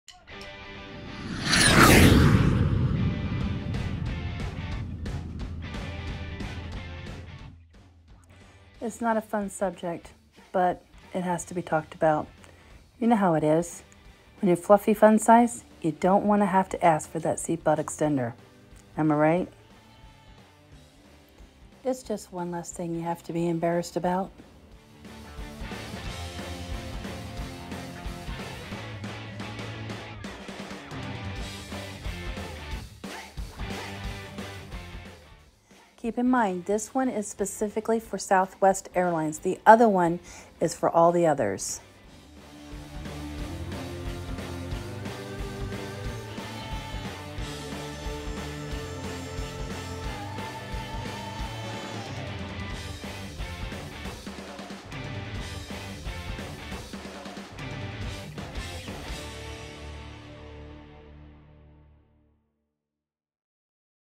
Airline Seat Belt Extenders Why?
Being Fluffy fun size traveling can be a bit stressful and sometimes embarrassing. I purchased these Airline approved (very important) seatbelt extenders. It’s just one less thing to stress about. Amazon double pack one low cost investment to have a little less stress while traveling.